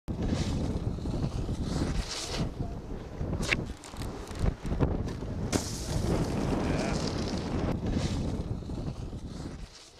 TTR 2Star NORGESCUP: Superstyle Session
GEILO: Sunday 19. December all hell breaks loose as we invite every ripping chick to come shred at Norway's only ALL GIRLS slopestyle competition in Geilolia Skisenter. The park is catered to all riders and we can promise it will be a great kickoff!!

Geilo IL, Geilolia Skisenter and the Norwegian Snowboard Association welcomes you to SuperStyleSession 2010.